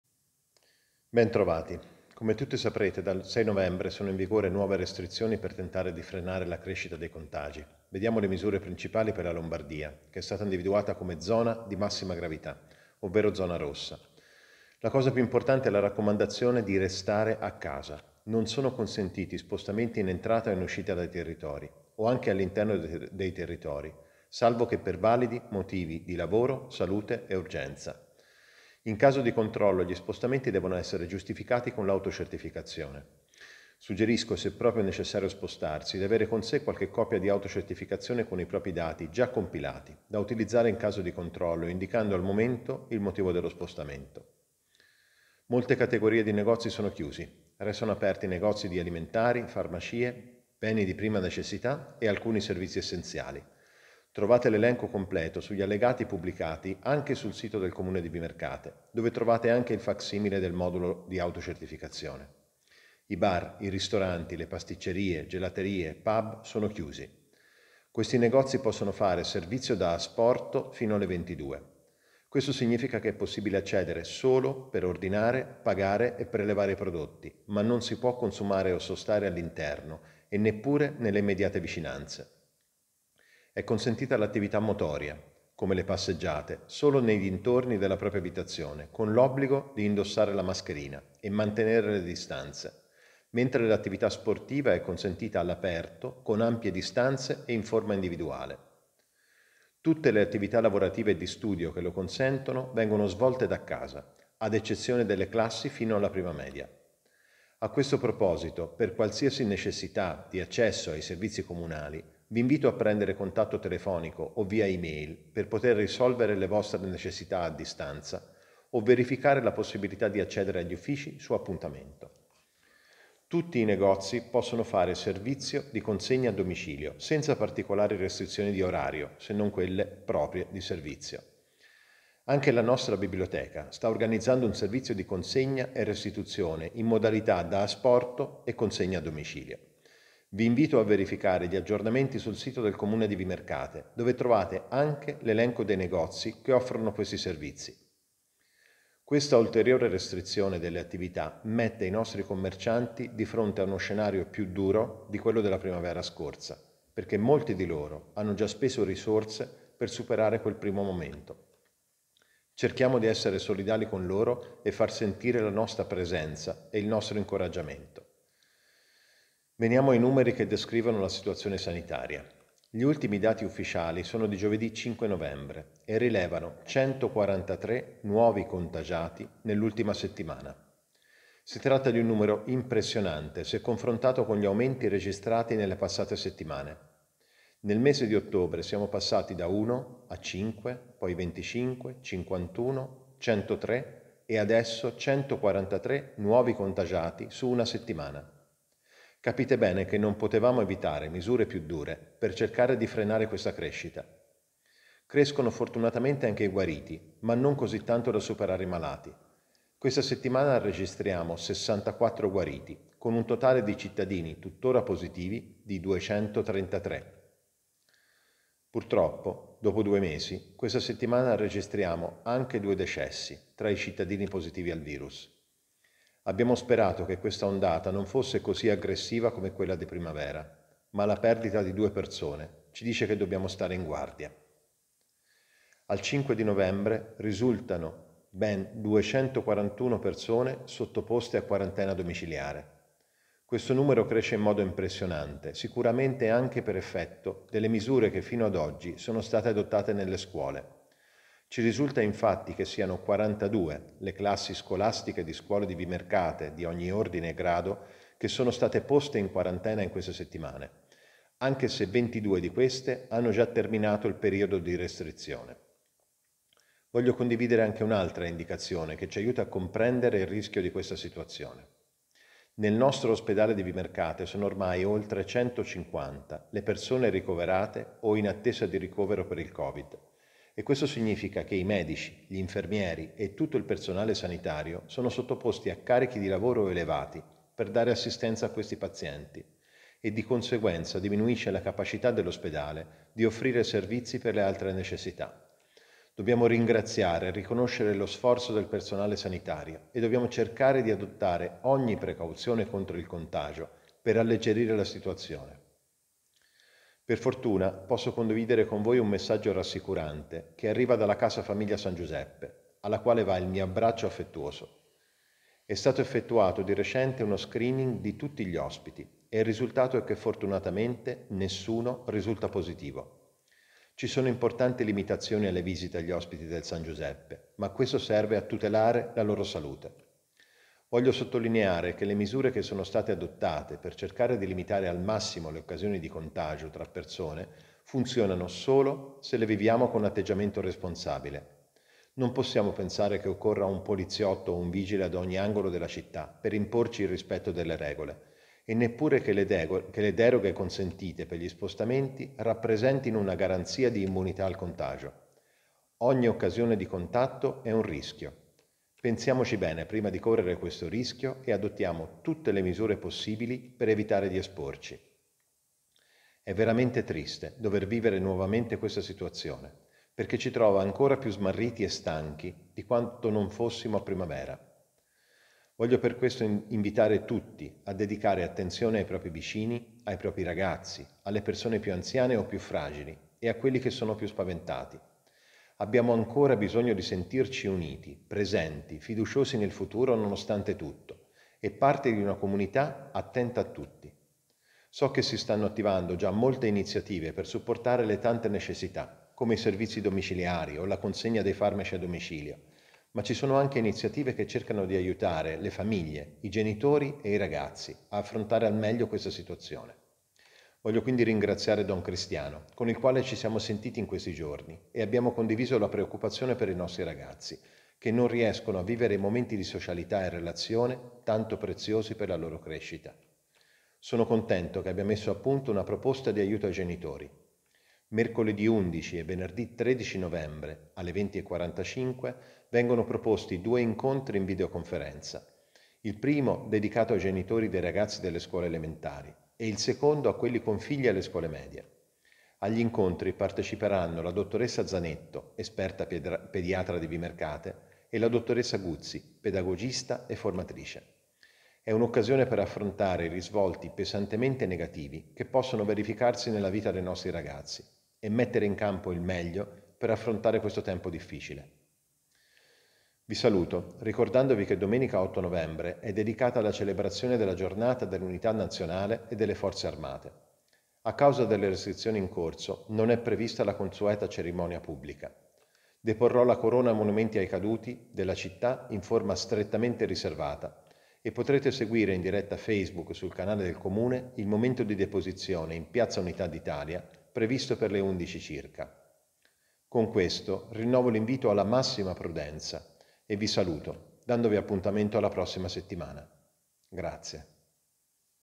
Videomessaggio del Sindaco di Vimercate Francesco Sartini di venerdì 6 novembre 2020
Il Sindaco aggiorna sui numeri del contagio e sulle restrizioni in vigore in Lombardia